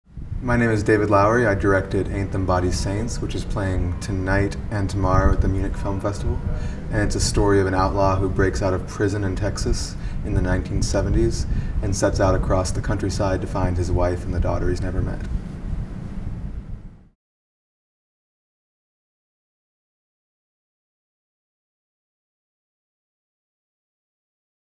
A Film By... | David Lowery
Auf dem FILMFEST MÜNCHEN 2013 in der Reihe 2013 zu sehen: David Lowerys neuster Film AIN'T THEM BODYS SAINT.

Kamera: Angelique Delibero, Ton: Erik Hartmann, Interviewer: Matthias Kreter